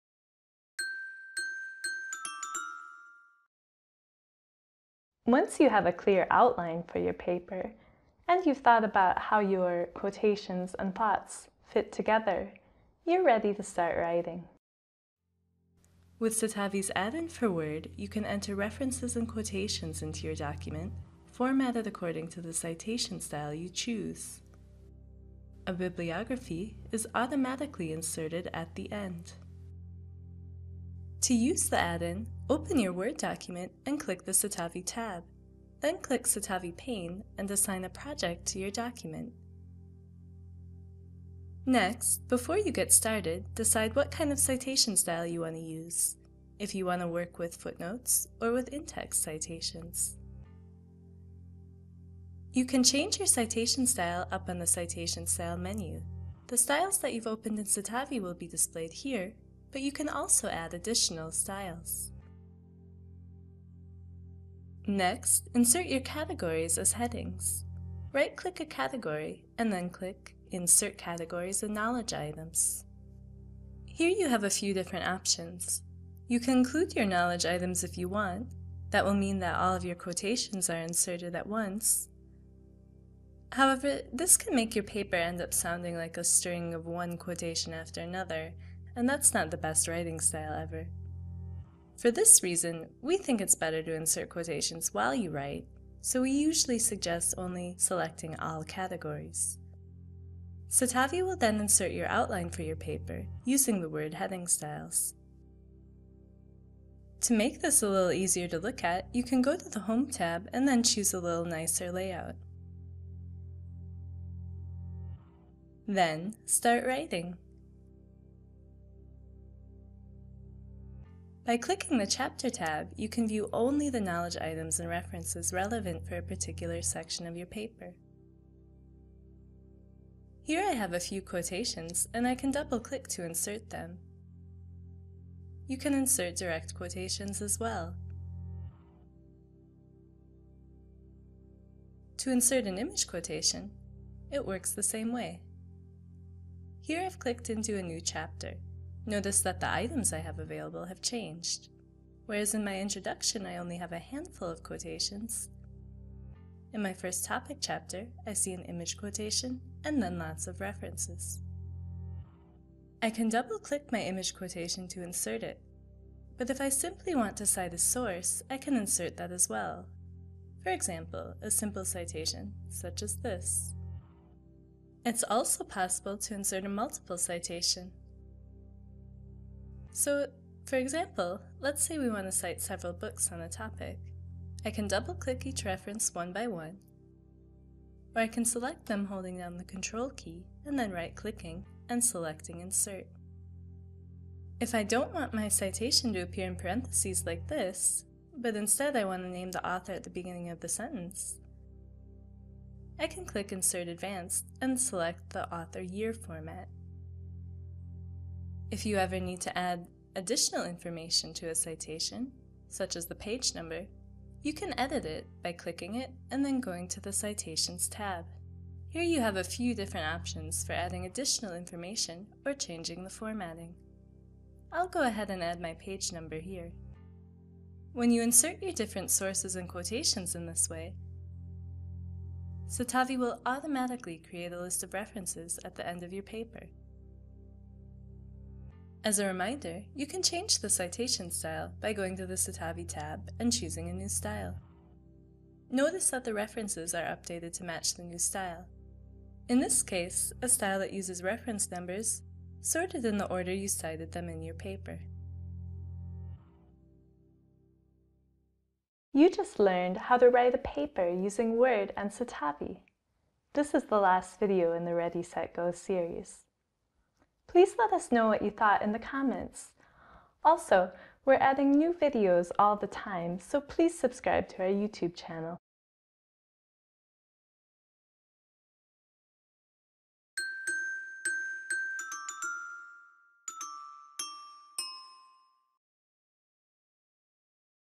8/8 Writing with Citavi 5 and Word
The moment you've been waiting for! Insert your category system into a Word document and then switch to chapter view to stay focused on the references, quotations, and ideas you want to use in a particular section. Citavi automatically formats your citations and bibliography in the style you select.